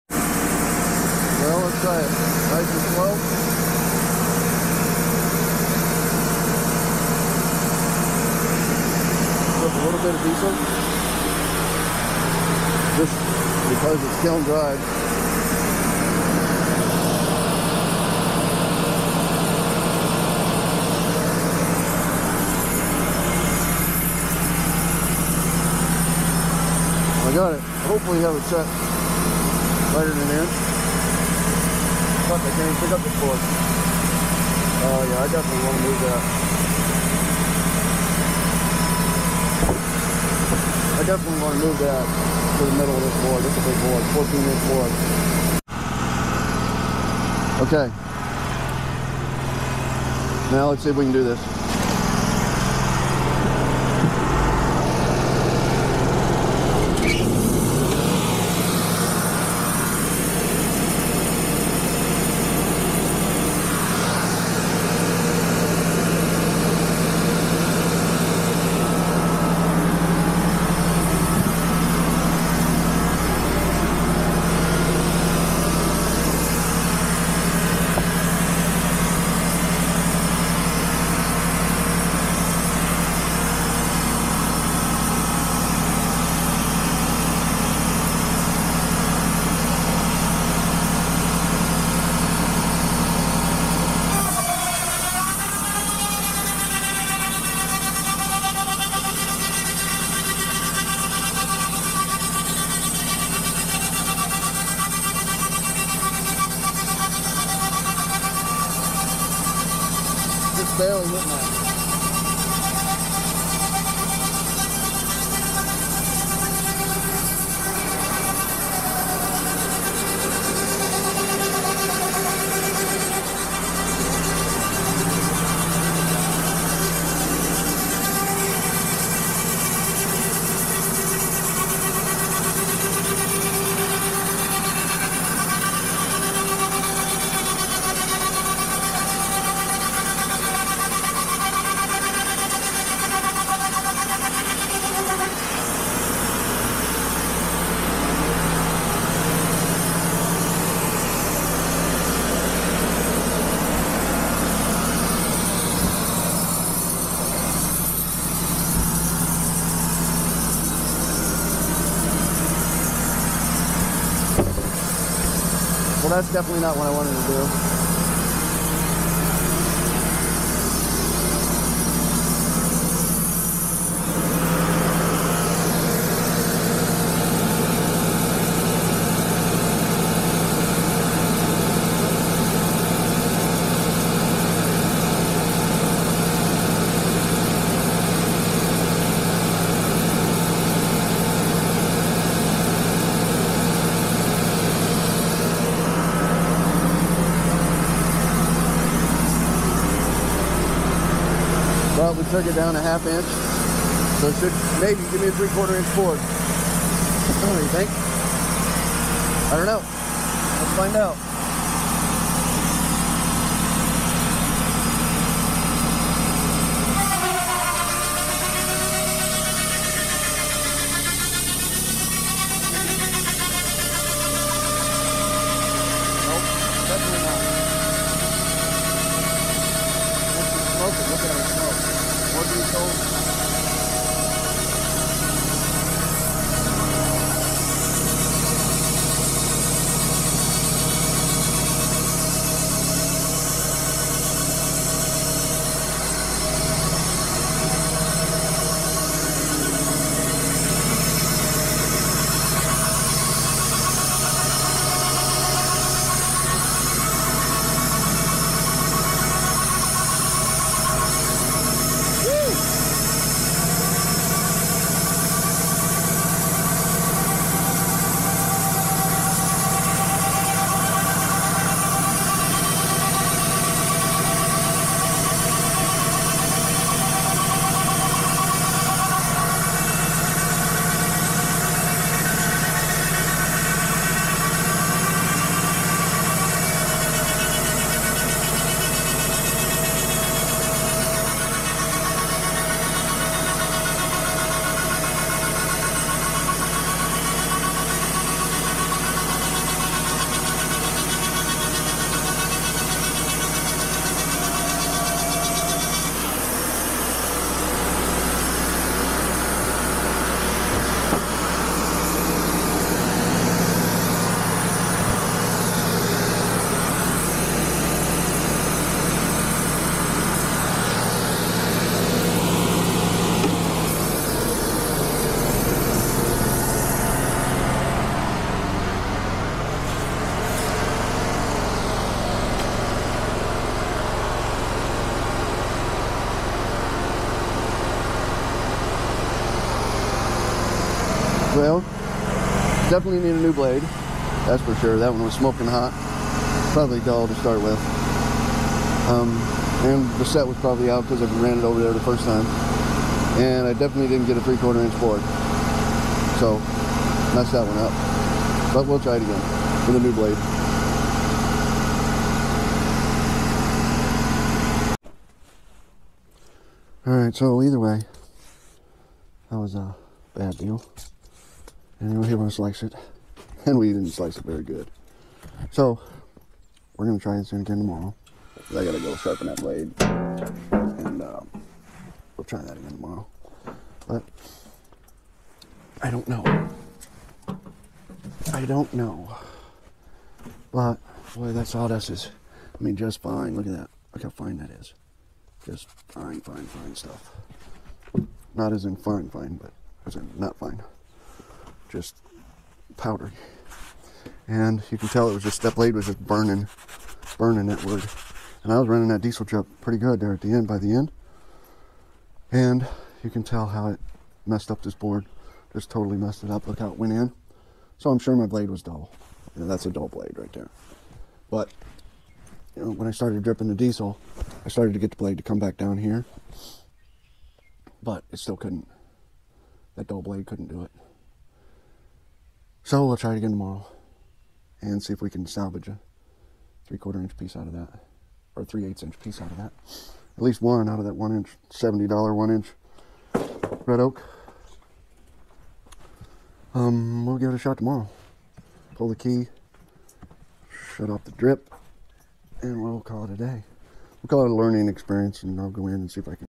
Resaw Attachment Wood Mizer LT35 bandsaw sawmill cutting first red oak 14 inch board
Resaw attachment on the sawmill. The Wood Mizer LT portable bandsaw mill with the resaw attachment of the first time.  I installed the resaw in the last video and this is the first board, Red oak 14" wide by 11' long.  All through the video i keep saying i am trying to get a 3/4 inch board and that is WRONG, I am trying to get 2 boards that are 3/8 inch thick.  This blade was very dull, I had just sawn a bunch of dirty, snow covered oak logs for firewood and I should have changed the Blade.   Also this resaw is NOT a "one man" operation.  I really need two people to run this correctly.  I also realize, Kiln dried red oak 14' wide is probably not what this Wood Mizer resaw attachment was designed to do.  I was really just trying to make sure i got the FIRST board on film.   I will say, I completed the order for 3/8 inch red oak and 3/8 inch kiln dried cherry boards.  I did not get the complete order on camera, i just needed to get the order out, no time for filming.  As always I hope you enjoy this video and I hope it helps someone out in some way.